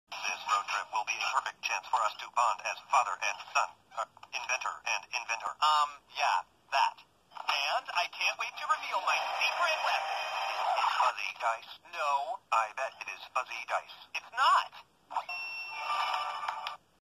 Bowser And Caesar Prepare For The Dumbest Road Trip In The World
Caesar will never envision his king as his adoptive parent. You can't make him. XD

I'm not telling you where the audio is originally from this time, by the way! You don't need to know, the source is terrible and boring, and I've never liked it, ever. Never as a kid when my mom would babysit for our neighbors, and especially not recently as stupid background noise while I'm working. :3